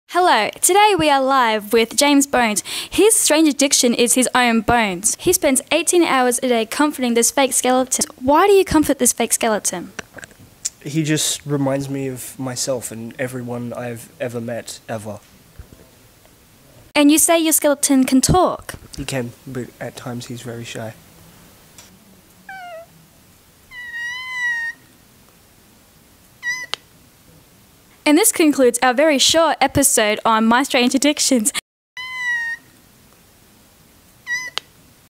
Quicktake: My Strange Addictions, Bones.
got given a task to make a funny mockumentary in 15 minutes.